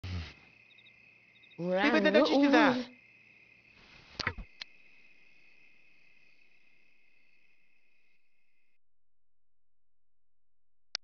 Denied!
This is a few seconds worth of video of Blaze turning down Lexia for sex. The guy saying no, thats rare! (This was waaay before the proposal)